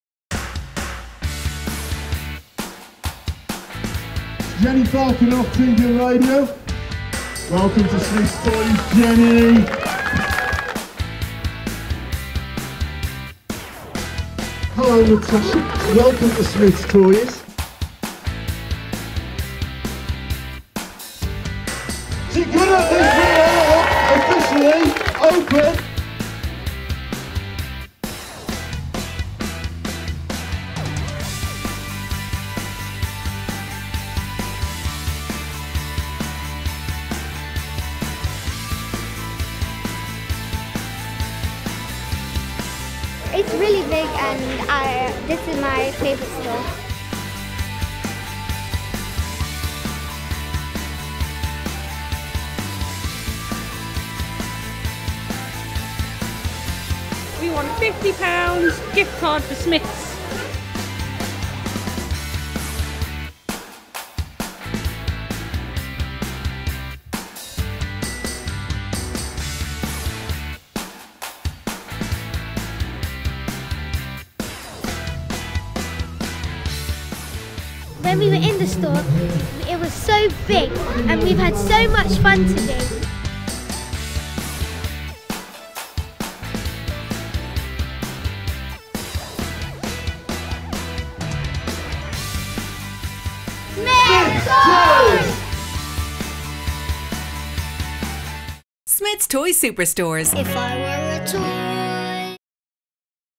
Smyths Toys - Store Opening Highlights - Staples Corner
We have officially opened a NEW Smyths Toys Superstore in Staples Corner Unit 1, Staples Corner Retail Park, Geron Way, London, NW2 6LW!

Here are some fun and exciting highlights from our store opening!

See below for any upcoming in-store events in a location near you.